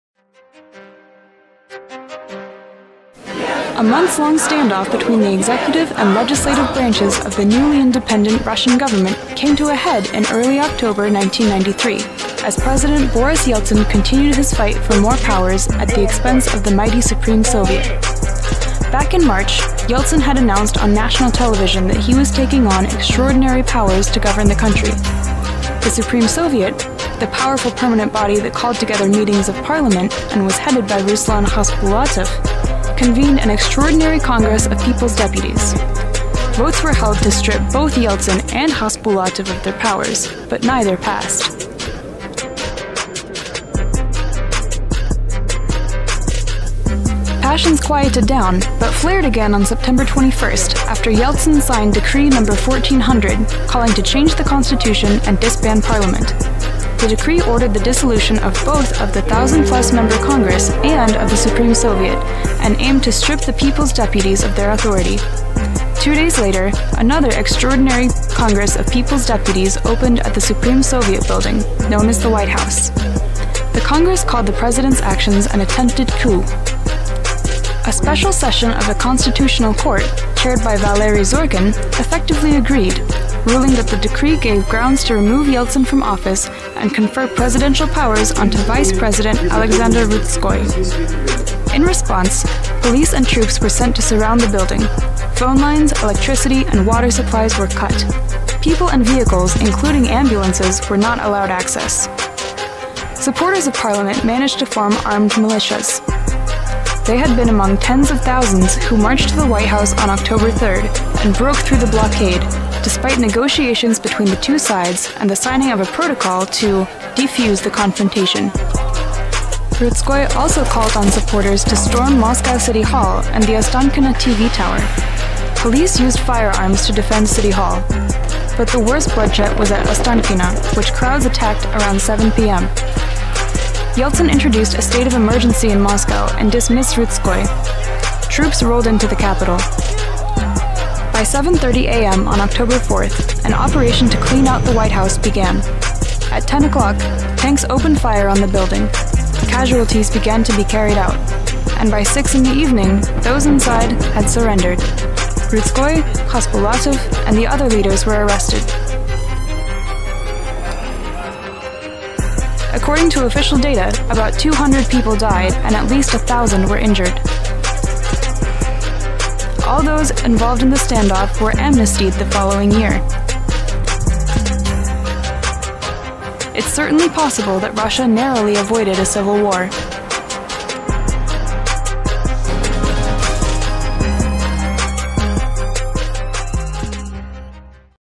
Black October 1993 On The Verge Of Civil War
On October 3-4, 1993, a stand-off between two branches of the newly independent Russian government -- the executive branch, represented by President Boris Yeltsin, and the legislative branch, represented by the chairman of the Supreme Soviet Ruslan Khasbulatov -- escalated into an armed conflict that left nearly 200 dead.